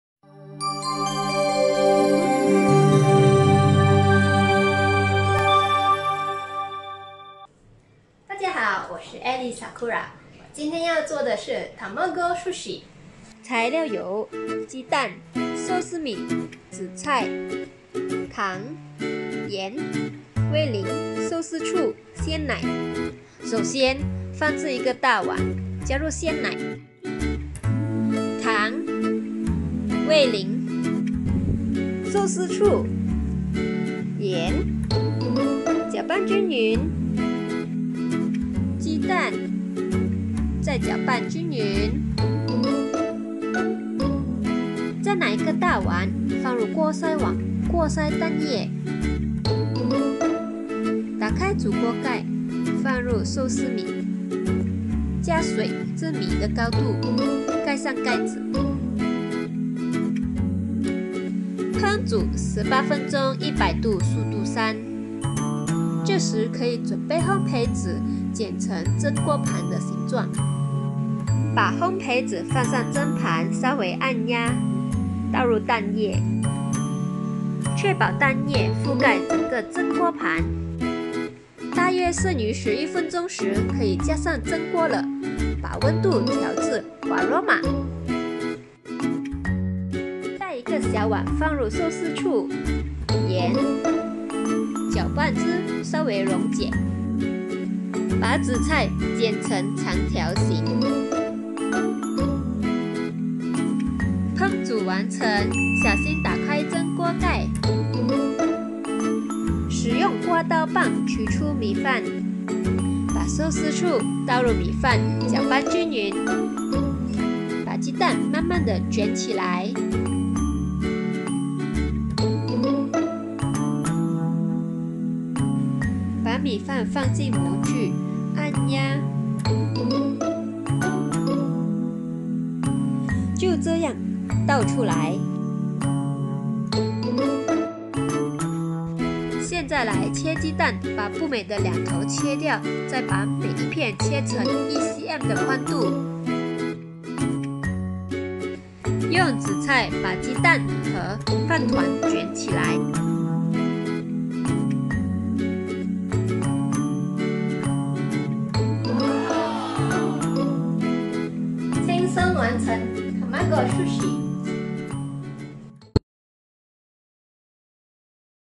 Thermomix 美善品 輕鬆做出 Tamago Sushi｜by Elly Sakura
░ 想要了解更多或購買 Thermomix 美善品

向小美殺手購買Thermomix 美善品就可以加入 小美殺手俱樂部 獲得更多使用小知識和食譜